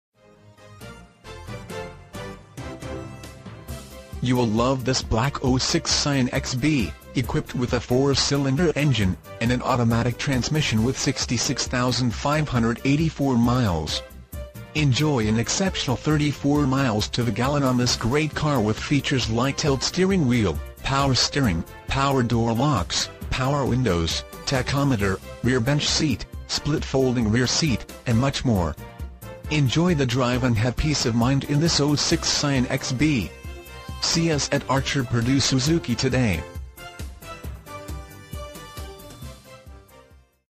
SOLD - 2006 Scion xB Archer-Perdue Suzuki
Archer-Perdue Suzuki

14227 S Street

2006 Scion xB

 66,584 Miles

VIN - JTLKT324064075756

Imagine driving this Black 2006 Scion xB, equipped with a 4 Cyl. engine  and an automatic transmission with  66,584 miles. Enjoy an exceptional 34 miles to the gallon on this great car with features like Tilt Steering Wheel, Power Steering, Power Door Locks, Power Windows, Tachometer, Rear Bench Seat, Split Folding Rear Seat, and much more. Enjoy the drive and have peace of mind in this 2006 Scion xB. See us at Archer-Perdue...